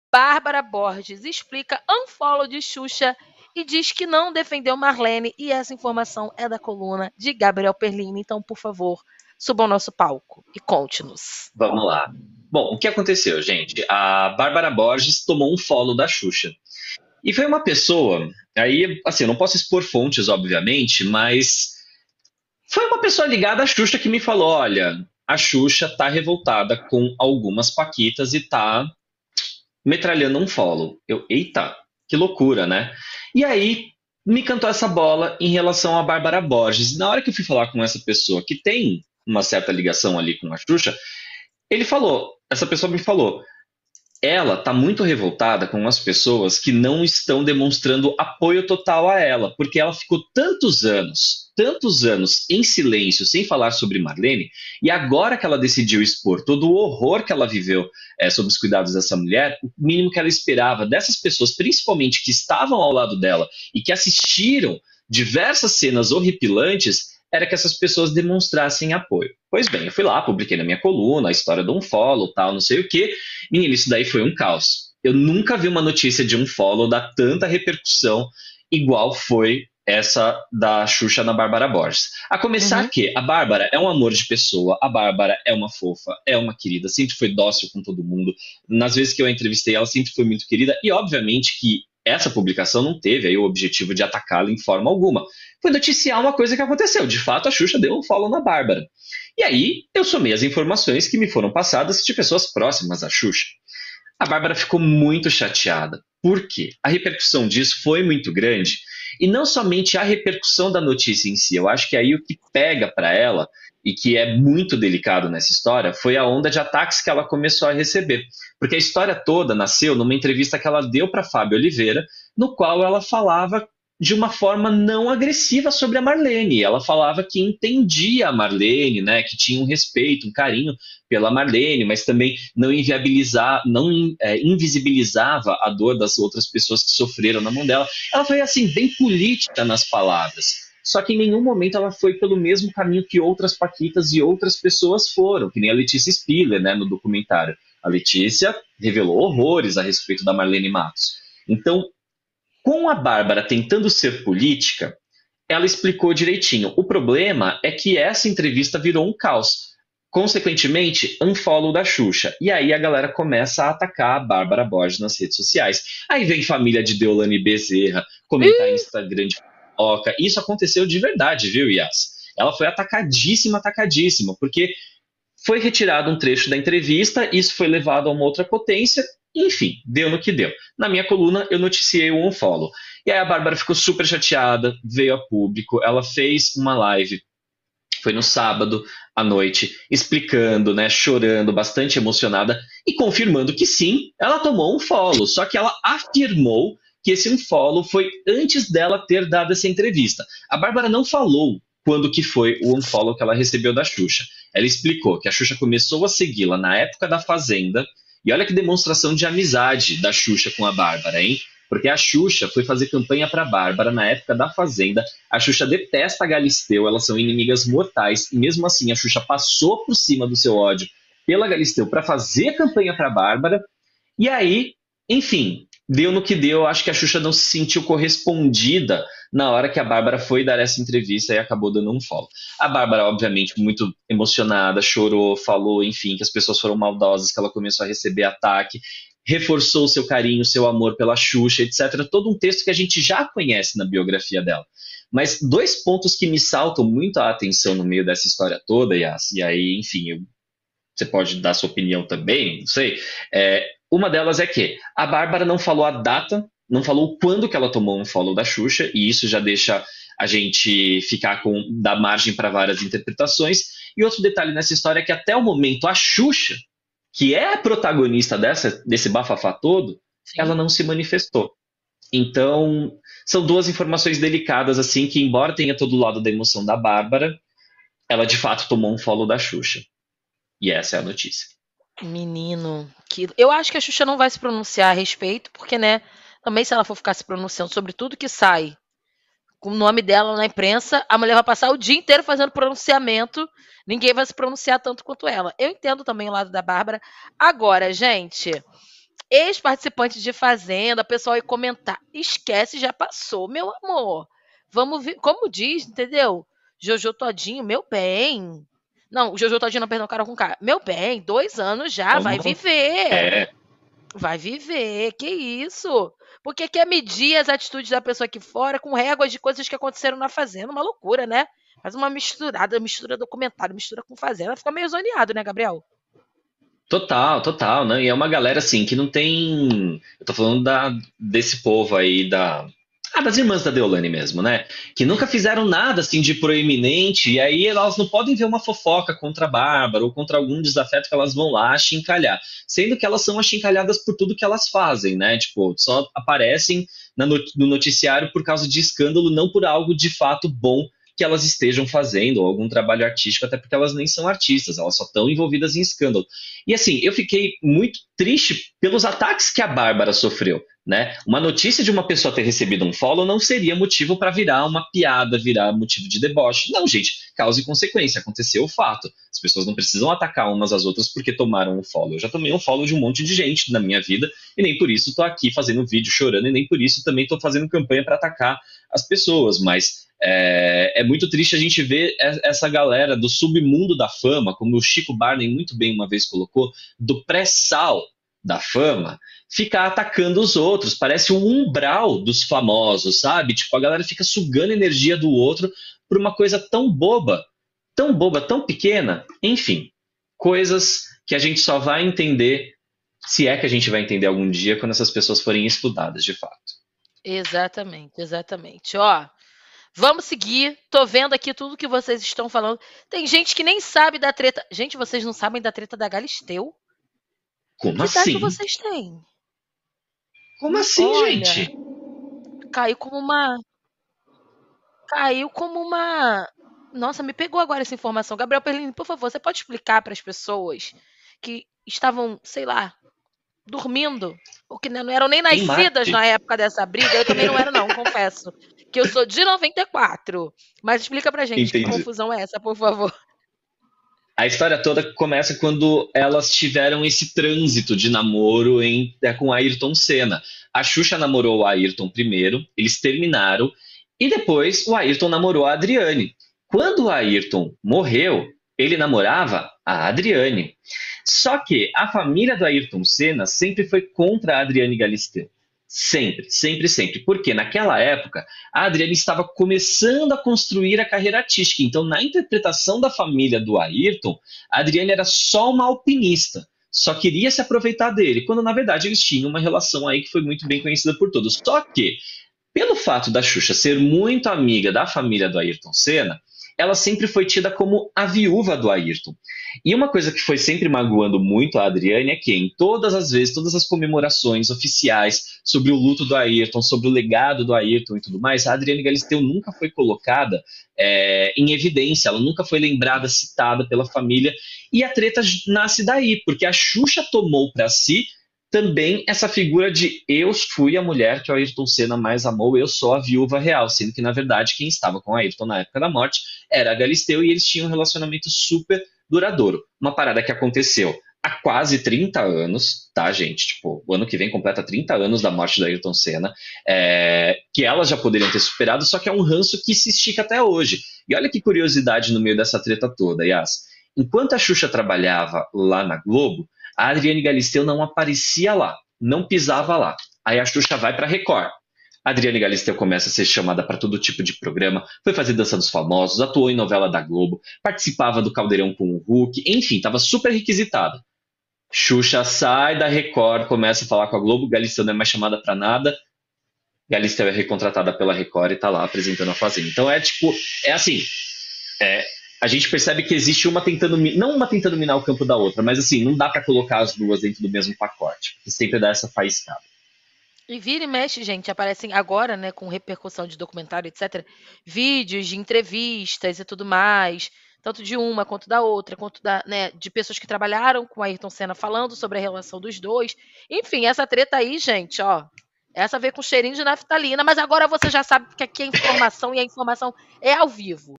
Bárbara Borges explica unfollow de Xuxa e diz que não defendeu Marlene Mattos após documentário
Recentemente, a campeã da Fazenda 14 deu uma entrevista e citou seu carinho por Marlene Mattos.